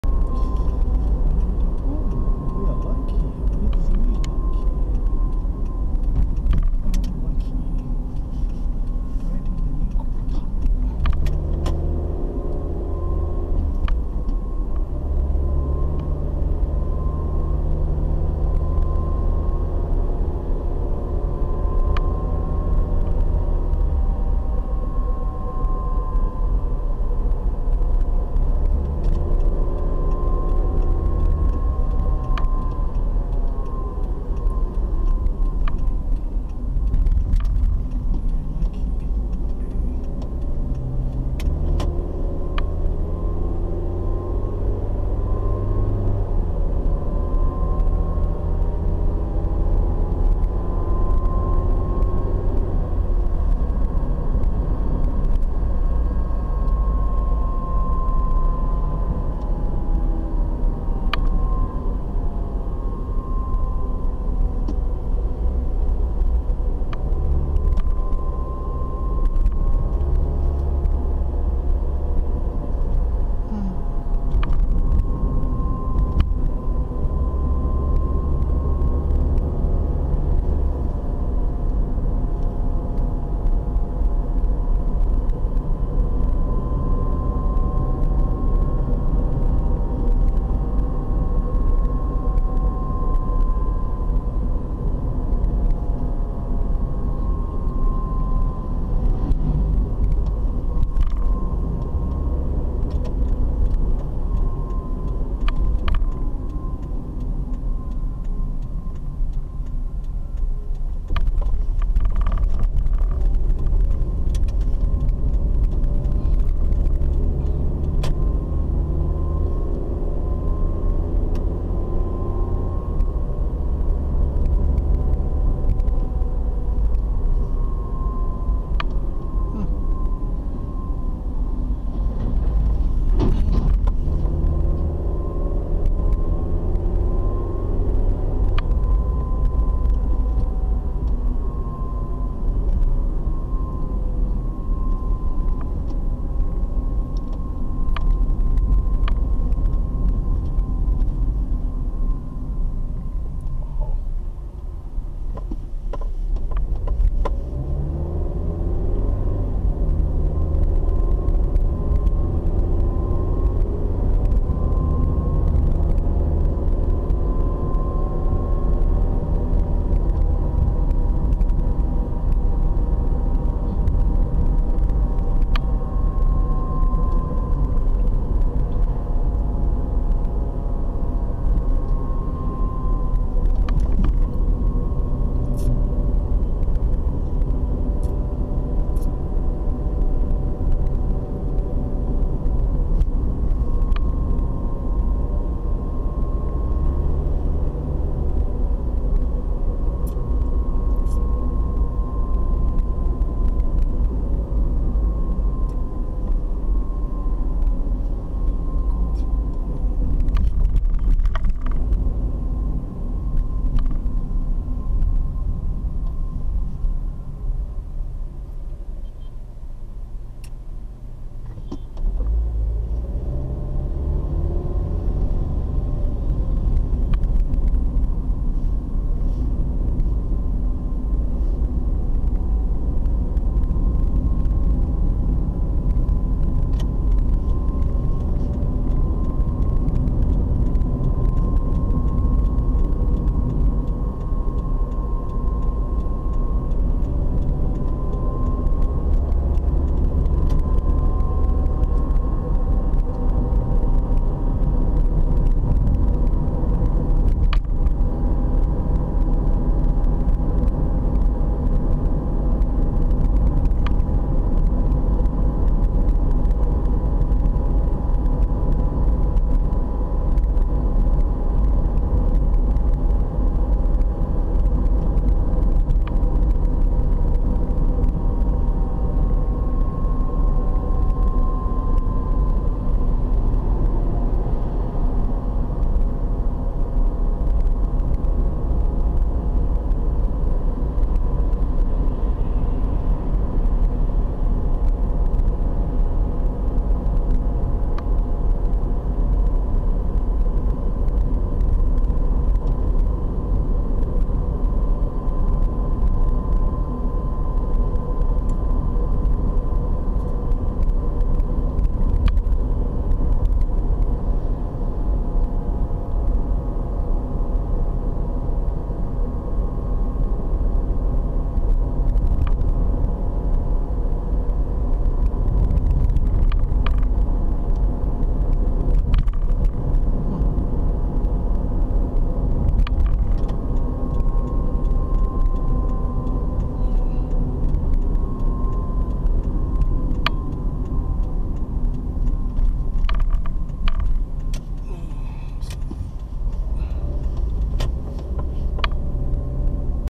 [Roads] Kudu Road → Chila Road → Los Angeles Boulevard → Nyerere Road | August 2021 | Lusaka Zambia
Driving through Los Angeles Boulevard, Nyerere Road, President's Lane, Ngumbo Road, United Nations Avenue and Nationalist Road, in Lusaka, Zambia.

This footage was taken on August 28 2021.

## Timeline
00:00:00 Alick Nkhata Road
00:00:10 Kudu Road
00:01:08 Chila Road
00:03:35 Kabulonga Roundabout
00:04:05 Los Angeles Boulevard
00:05:45 Nyerere Road

## Footage Metadata
* Time: 09H40--15H00
* Date: August 28 2021